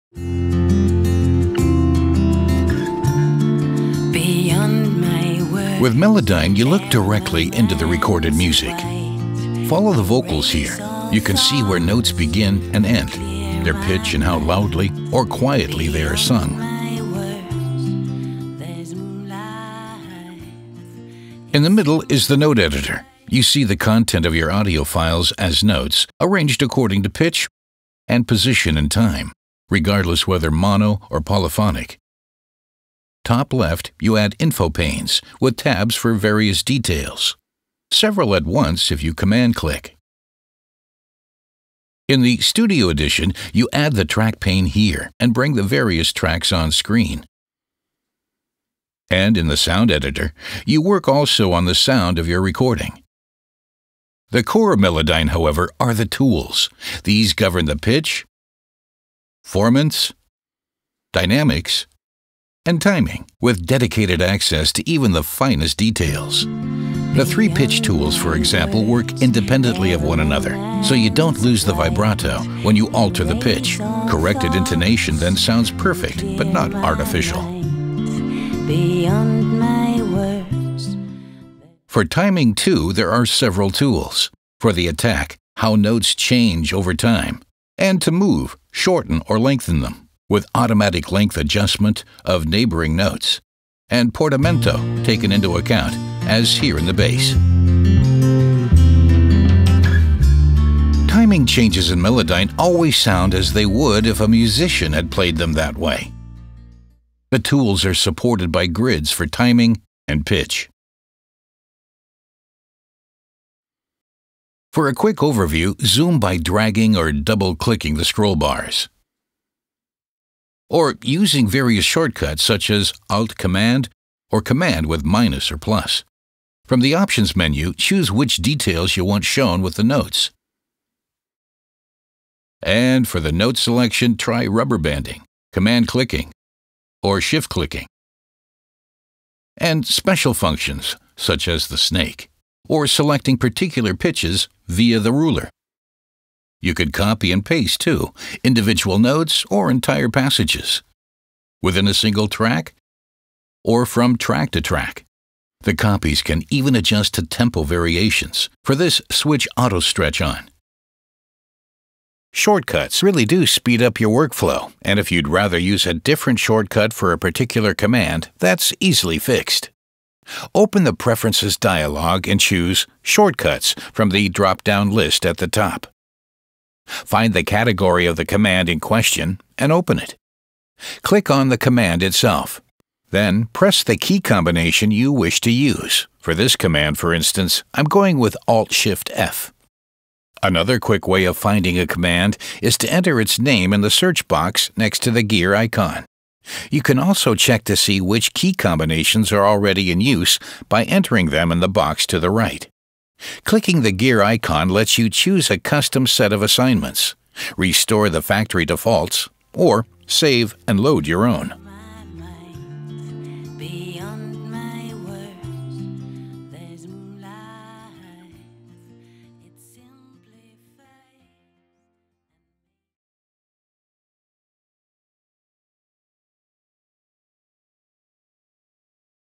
Melodyne 5: The basic workflow
In this film, we explain step by step how to work with the Grammy Award-winning audio software Melodyne that is used all over the world every day: a studio standard, in fact. For example, learn how to use the tools that govern pitch, formants, dynamics and timing with dedicated access to even the finest details. And there is much more to discover in this quick tour through Melodyne 5!

Why not try Melodyne 30 days for free, without any limitations?